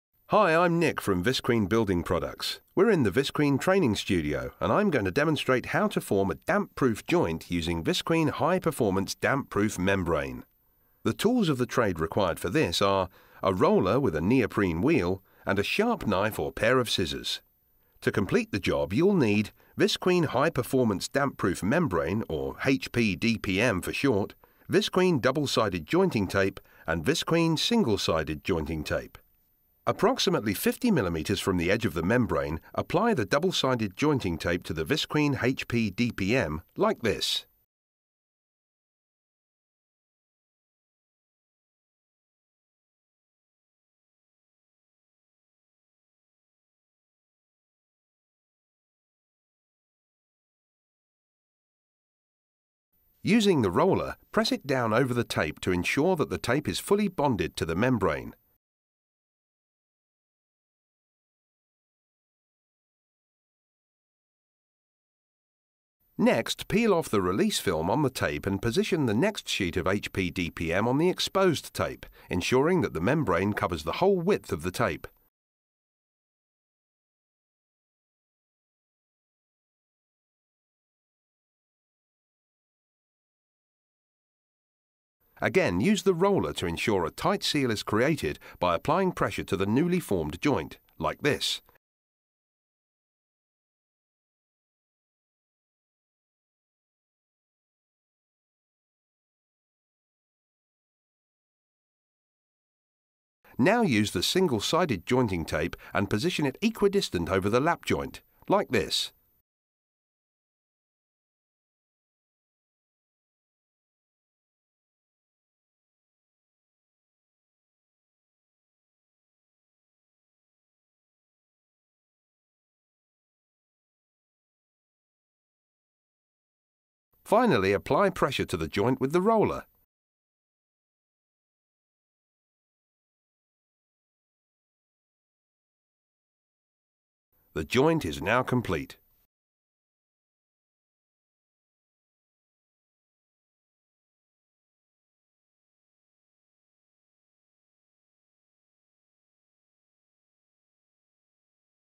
Jointing a membrane with Visqueen HP DPM
Visqueen High Performance DPM - BBA Approved -  is Visqueen's premium grade Damp Proof Membrane manufactured from prime virgin polymers for use as a Type “A” damp proof membrane as defined by BS EN 13967: 2012. The DPM is yellow in colour and available centre folded in 4m x 12.5m x 500um roll format.

BBA Certified
Visqueen's premium grade Damp Proof Membrane
Suitable for use on commercial and domestic applications
Comprehensively independently tested
Supplied centre folded to reduce the risk of cracks in screed

Visqueen High Performance DPM is suitable for applications such as heavy duty raft foundations, steel reinforced slabs etc where a durable, robust and high puncture resistant membrane is required.